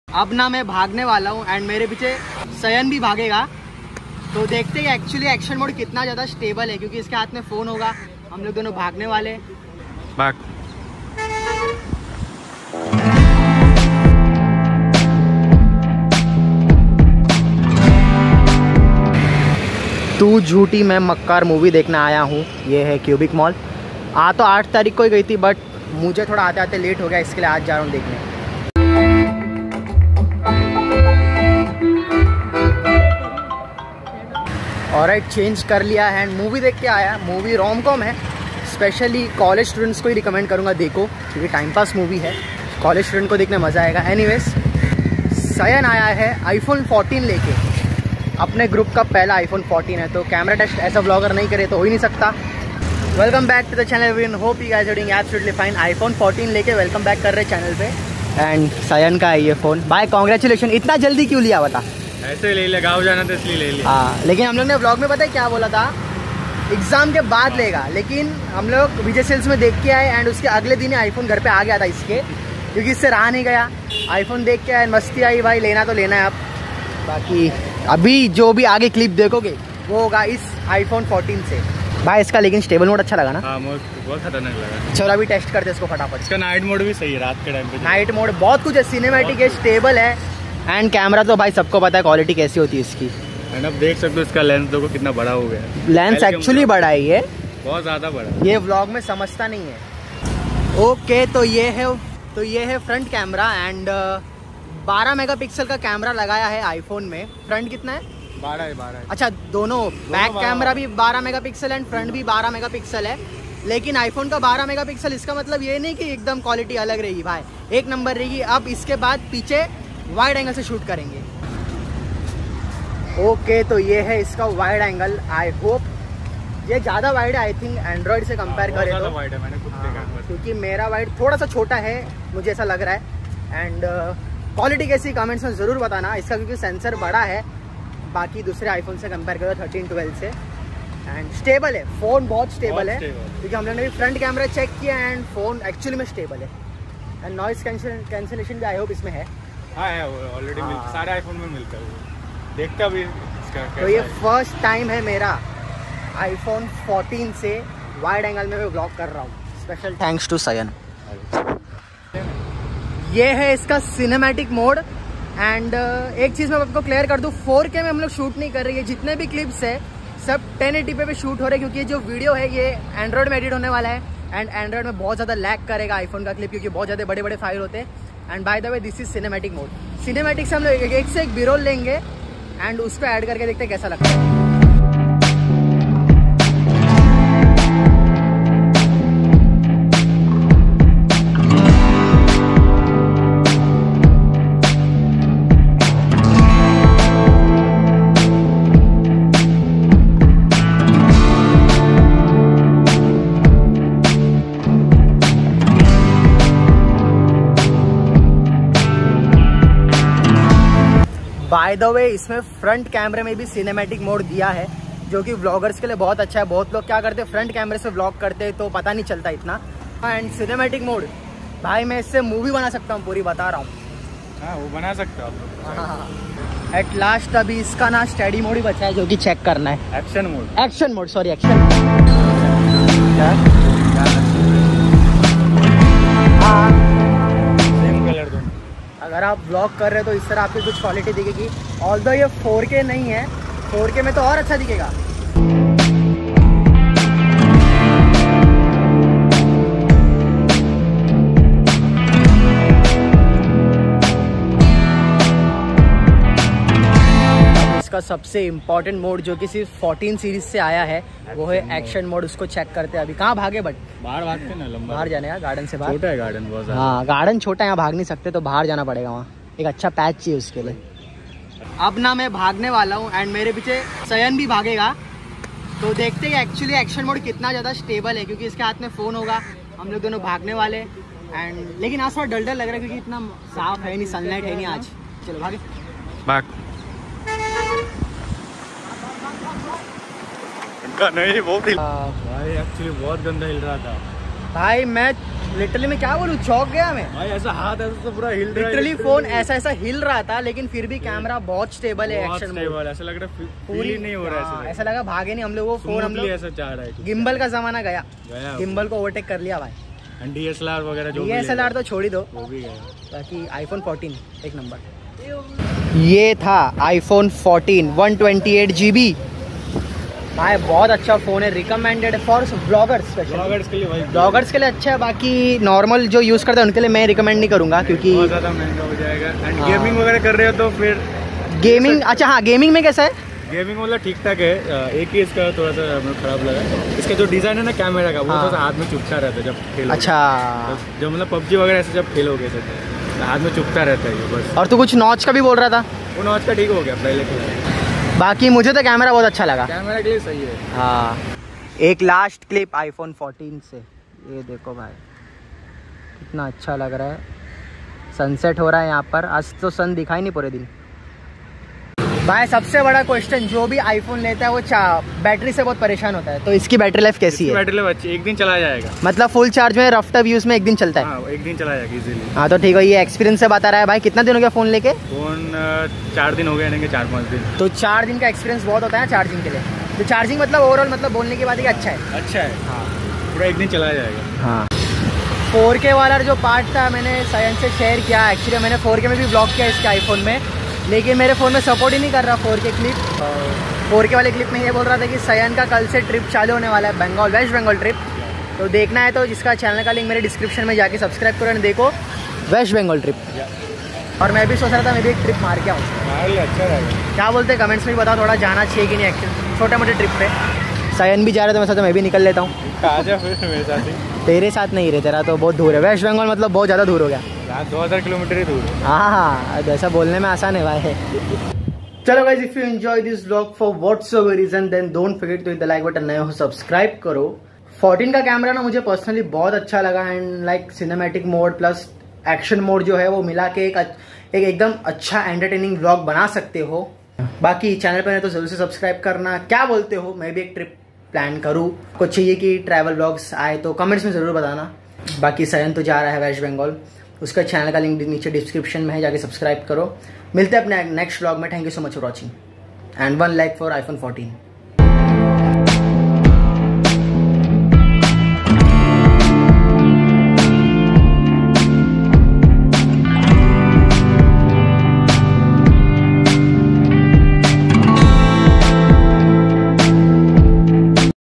Vlog With ( IPHONE 14 ) Cinematic And Action Mode Test | camera Test
In this vlog I have tested iphone 14 camera
Cinematic And Action Mode has been tested and it's amazing

iphone 14  , Cinematic , Action Mode